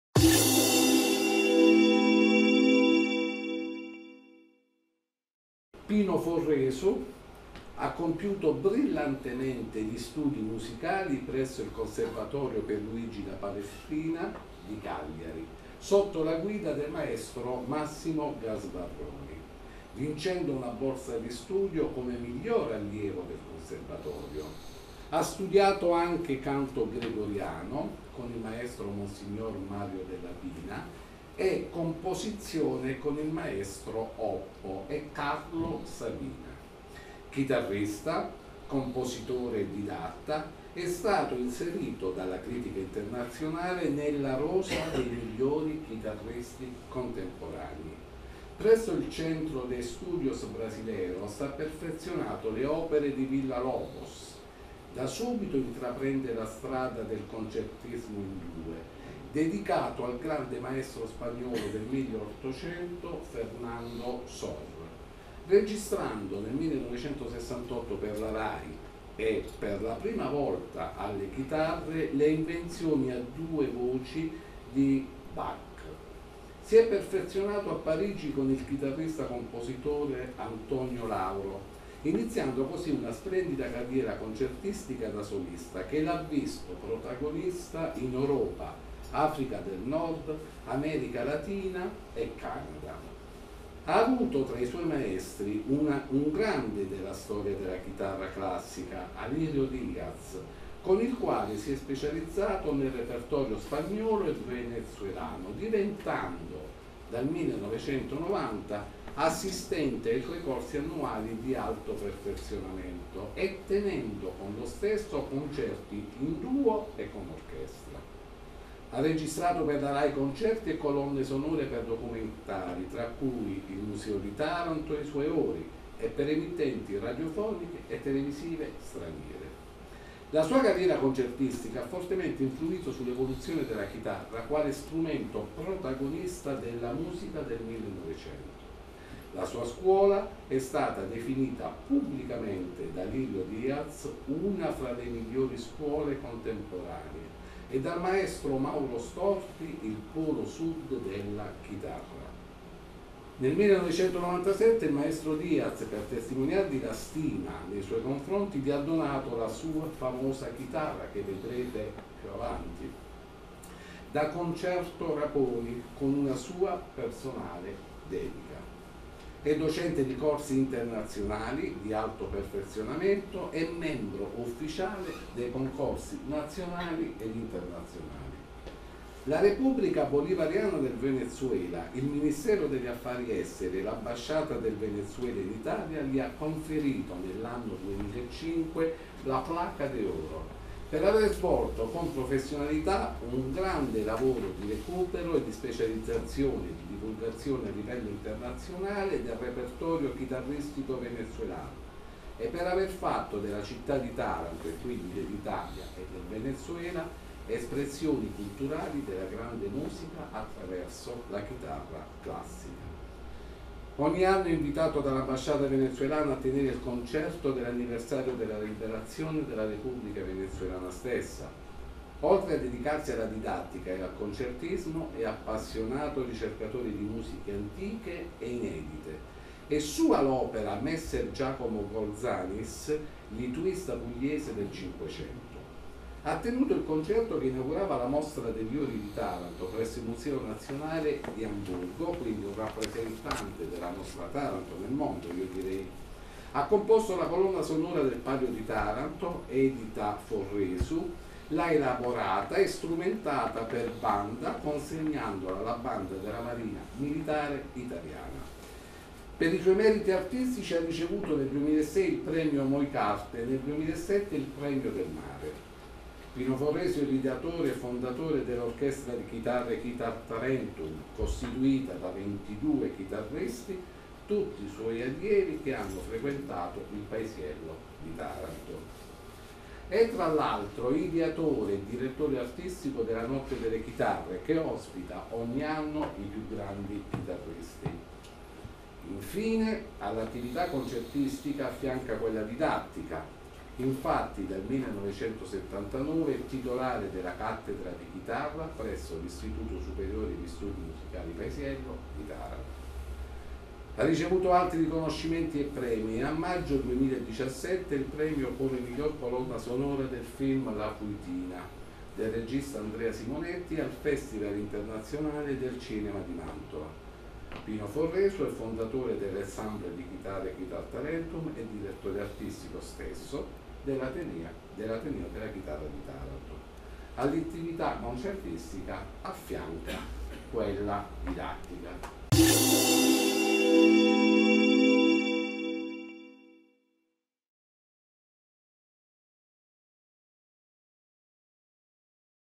LA BIBLIOTECA DELLE EMOZIONI. LA BIOGRAFIA DI PINO FORRESU
Ariel Signorelli legge la biografia del Chitarrista Pino Forresu.